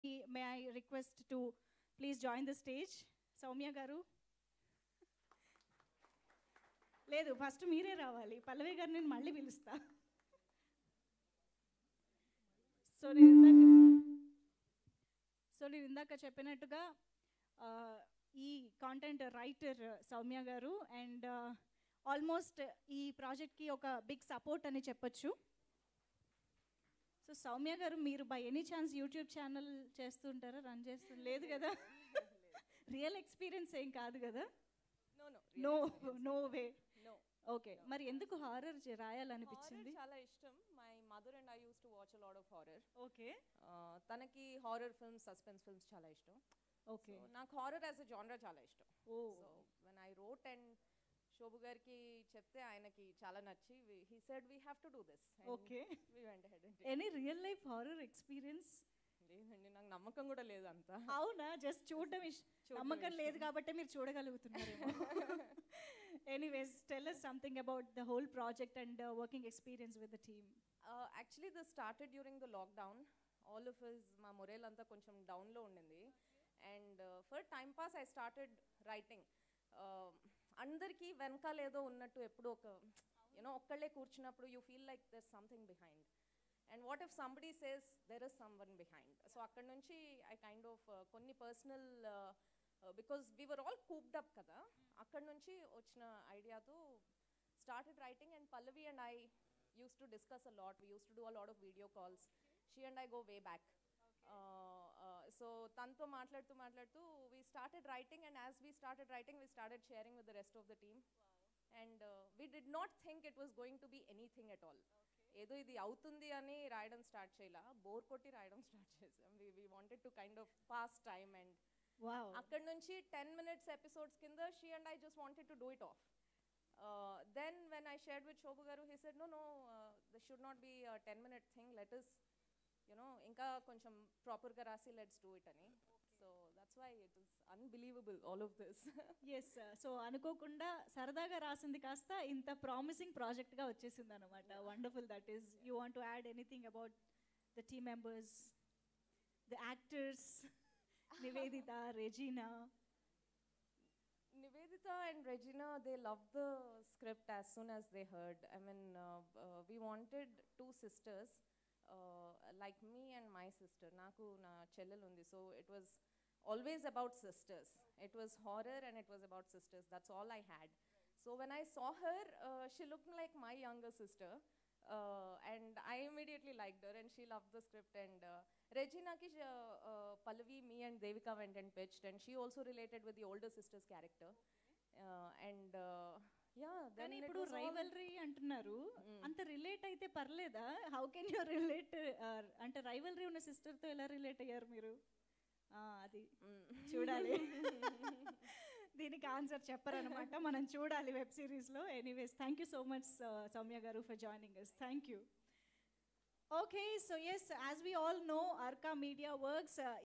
అనుకోకుండా సరదాగా రాసిన కథ..! | Writer Sowmya Sharma Speech at Anya's Tutorial Trailer Launch Event
అనుకోకుండా సరదాగా రాసిన కథ..! | Writer Sowmya Sharma Speech at Anya's Tutorial Trailer Launch Event | Vanitha TV